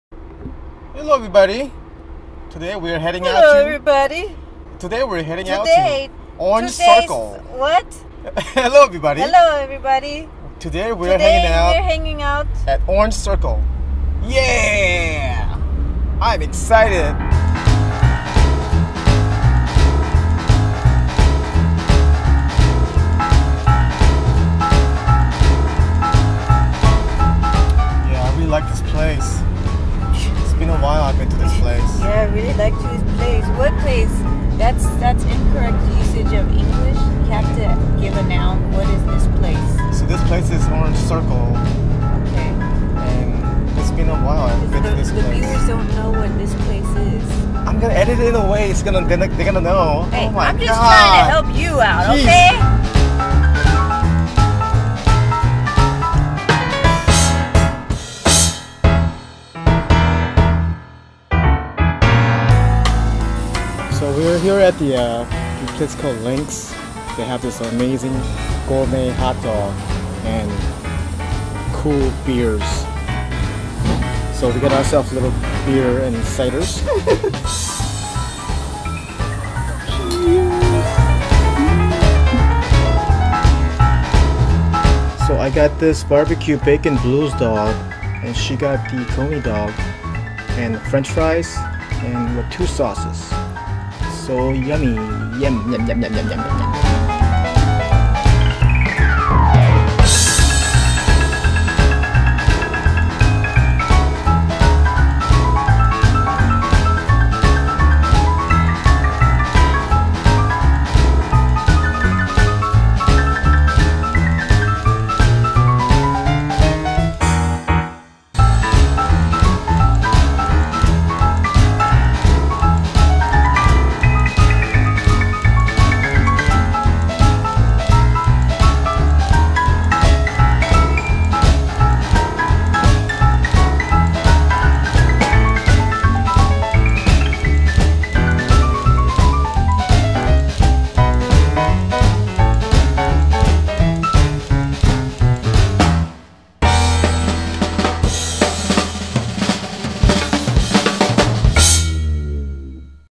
Hanging out at Orange Circle / Old Town Orange (VLOG#10)
In this episode, we were hanging out at Orange Circle or Old Town Orange, California.  We had dinner at gourmet hot dog place called Linx Dogs in Orange, CA.  We had  a great time there.

Equipment used:
Canon T3i
iPhone 6